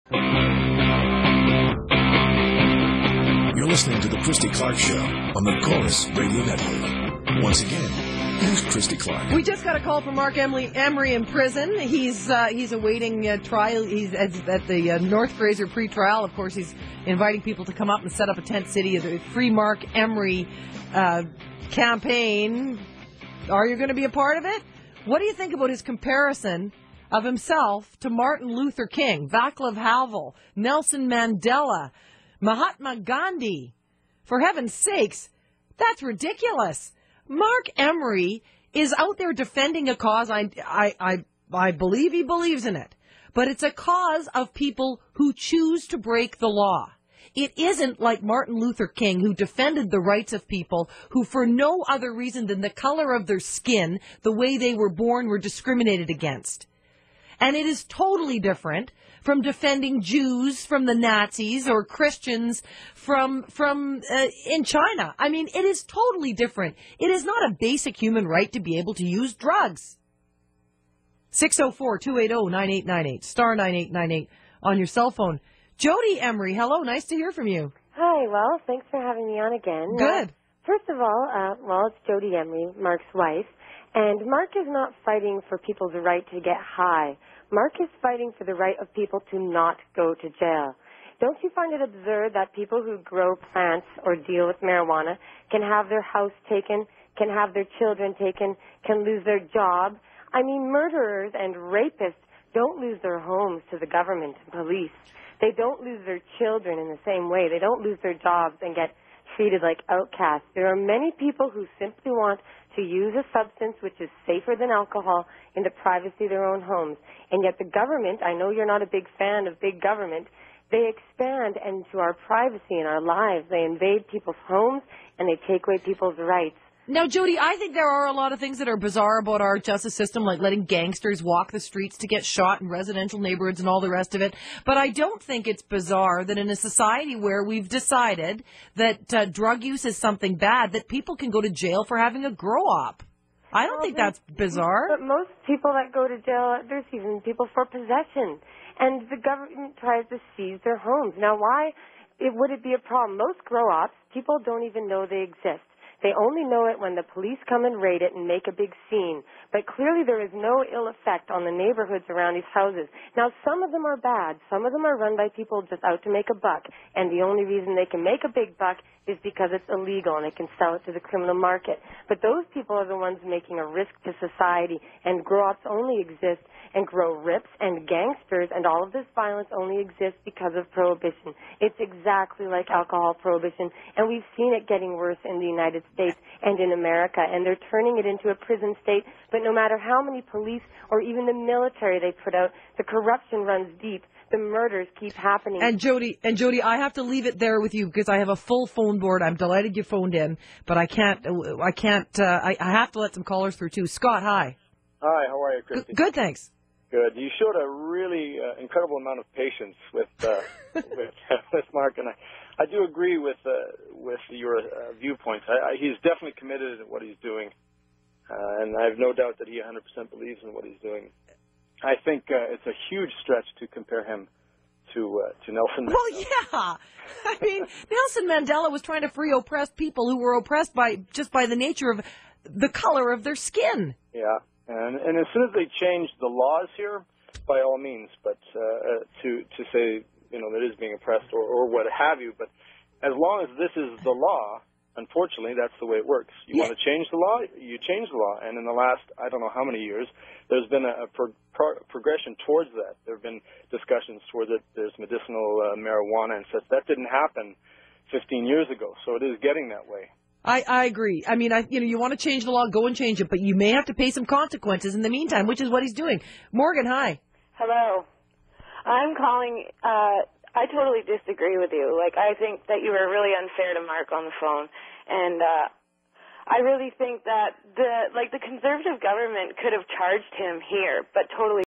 (2009, October 2nd) CKNW - Christy Clark Interviews Marc Emery from Jail [2 of 2]
Note:  I have no association with Marc Emery or Jodie Emery besides the fact that I wish I had their courage.

I was quite offended myself by Christy Clark's behaviour and comments. I can only imagine how the folks in Vansterdam felt. She came off as having an overly enlarged ego. Is she always like that?

She did get me thinking, could she be possibly correct with her insinuation that Marc Emery cannot be kept in the same category as MLK, Gandhi, Nelson Mandela or even Jesus? I bet you she was oblivious to the fact that Jesus can be placed in the same category. He also practiced non-violent resistance and civil disobedience to help achieve his political goals. She was very uneducated on the reasons why these great humans were compelled to do what they did.

Do you know who my heroes are? My heroes are those who feel compelled to stop injustice by taking on the burden of the injustice themselves, highlight it for others to see and compel them to help make the necessary changes to stop it. THOSE are my heroes.

Marc Emery, MLK, Gandhi, Nelson Mandela, Jesus and David Malmo-Levine are ALL among my heroes. Christy Clark should think twice about voicing opinions on subjects she doesn't know very much about for fear of looking like a bigoted jack-ass.